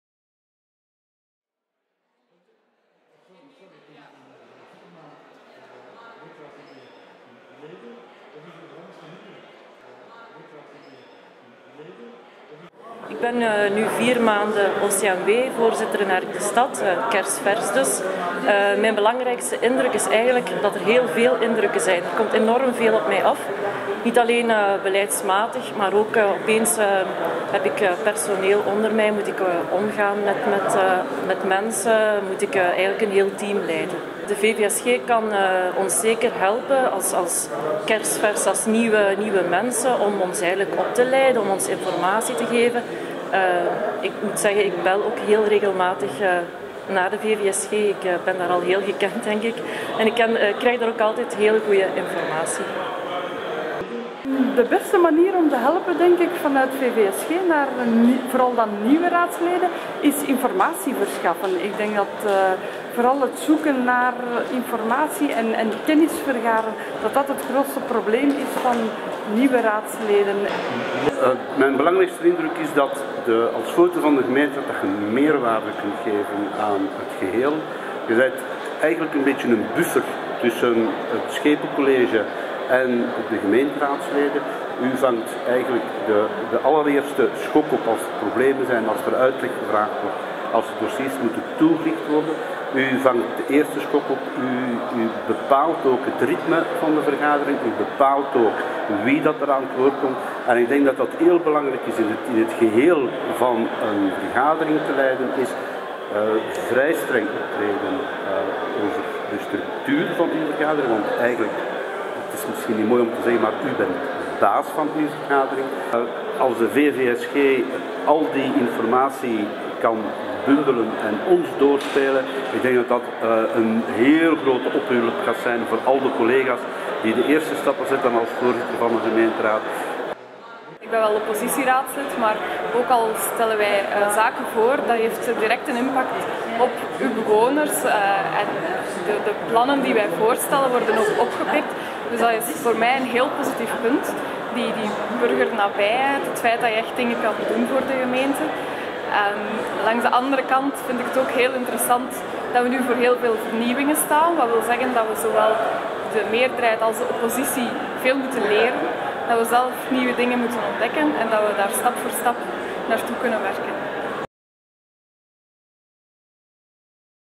Raadsleden over hun ervaringen na 1 kwartaal in gemeente- of OCMW-raad
Filmpje naar aanleiding van de VVSG-raadsledenavonden.